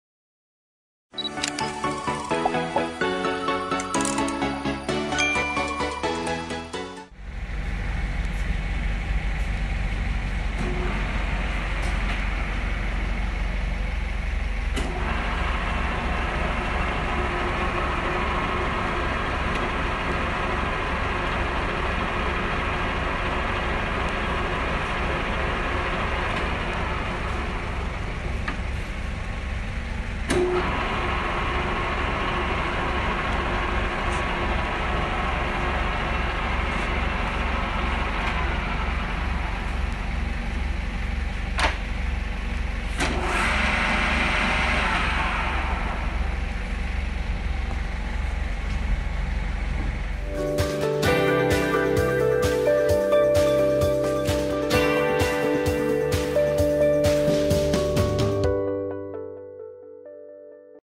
A la venta Taladro Cincinnati 24| Industrias Frimar | Maquinaria
Tipo Banco
Cono MT3
Mesa 23"
Carrera 12"
Garganta 10"
HP 5
Capacidad de perforación 2-1/2"
Voltaje 220  | 3 Fases
Peso 3,500 lbs
Dimensiones 24" x 56" x 104"